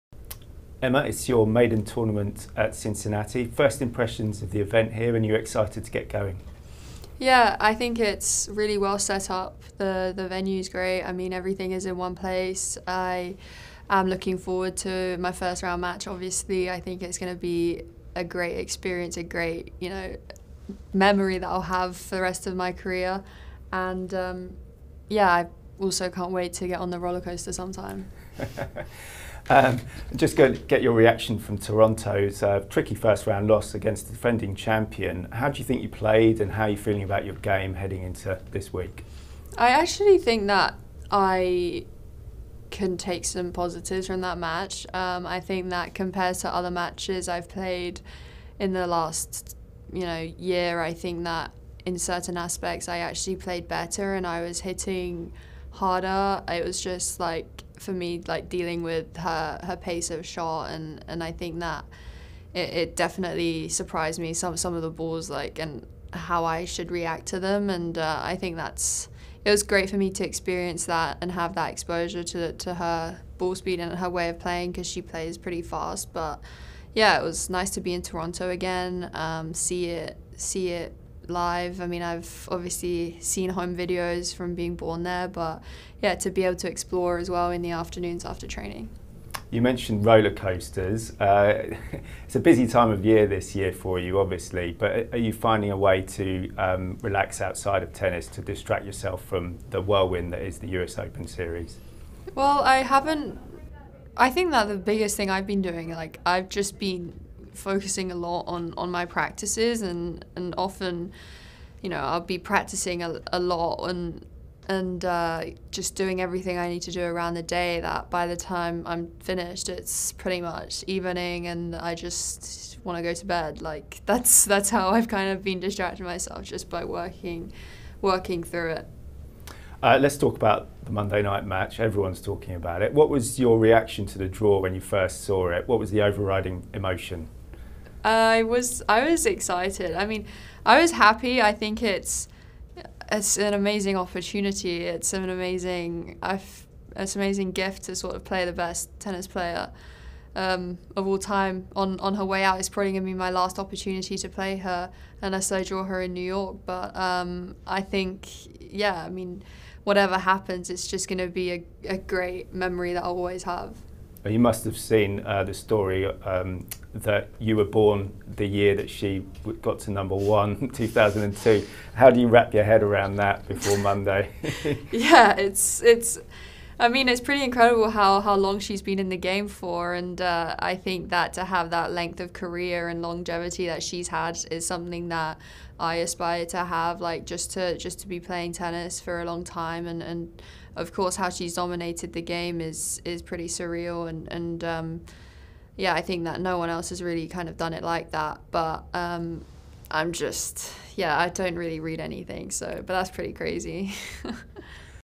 Pre-Tournament Interview with Emma Raducanu at the Western & Southern Open
Video from WTA